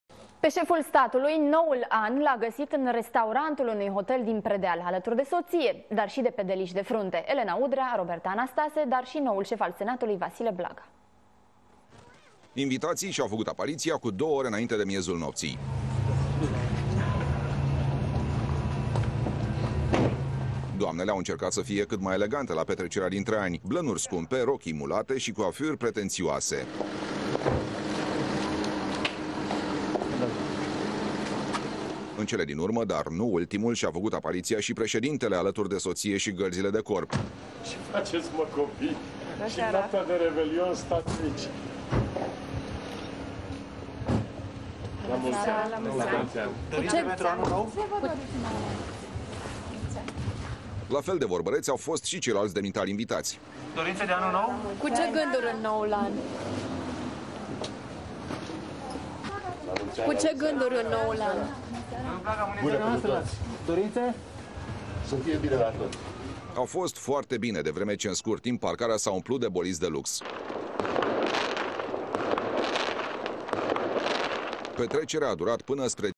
Traian Basescu a petrecut Revelionul la munte 1 IANUARIE 2012
Presedintele a petrecut la trecerea dintre ani la munte. Alaturi i-a fost sotia Elena Udrea Roberta Anastase dar si noul sef al Senatului.